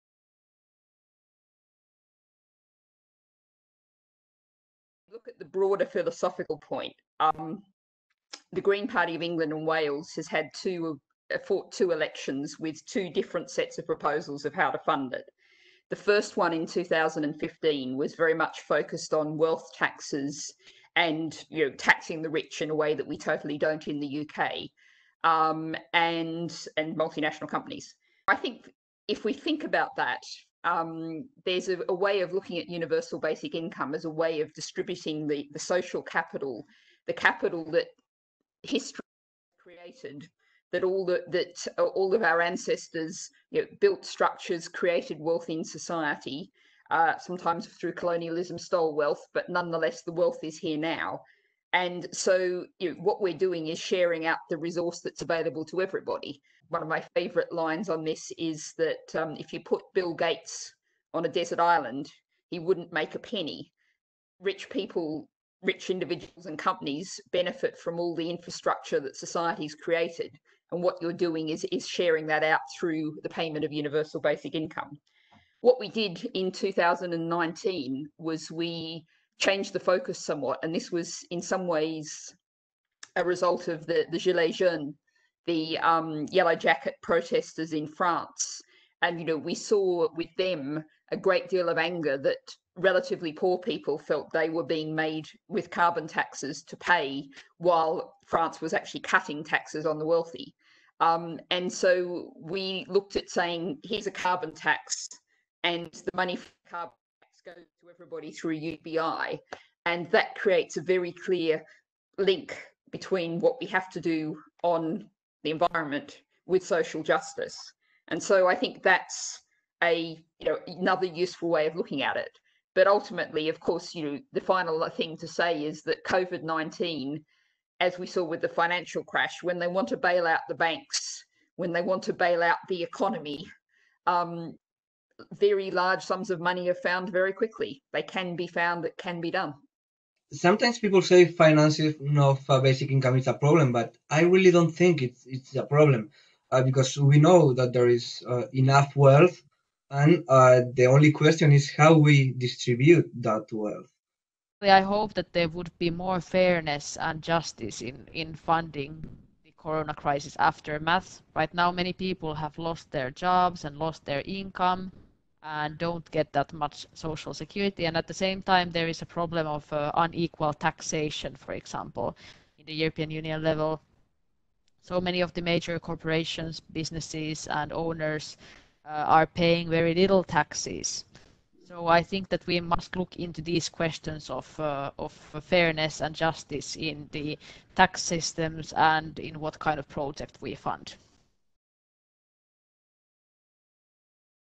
Universal Basic Income debate in the UK
"There's a way of looking at UniversalBasicIncome as a way of distributing social capital, the capital that history created." - Natalie Bennett, Green Party Member of the House of Lords.

If you are enjoying our summaries of the Post-Corona Talks, please consider making a small donation